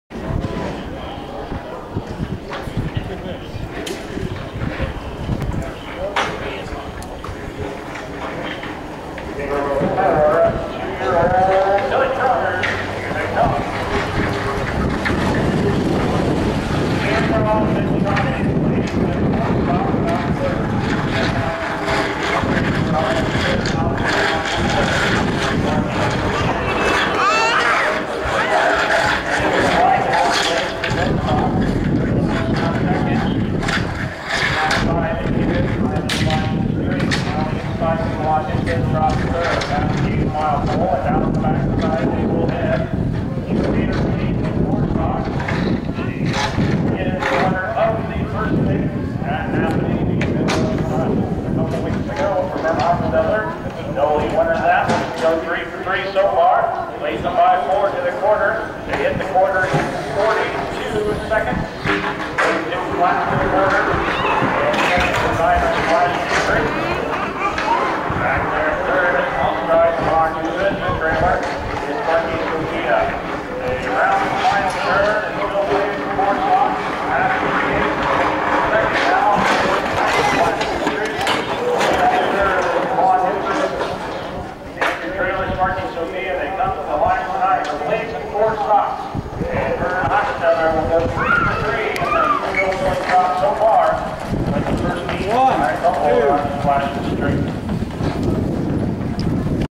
Goshen 9-1-17 ITPA 2yo Filly Trot 1st Heat Blazenfoursocks wins in 1:18.2
Goshen 9-1-17 ITPA 2yo Filly Trot 1st Heat Blazenfoursocks wins in 1:18.2, Ima's Flashen Streak 2nd in 1:19, The Blonde Dufus 3rd in 1:32.1, Sparky's Sophia 4th.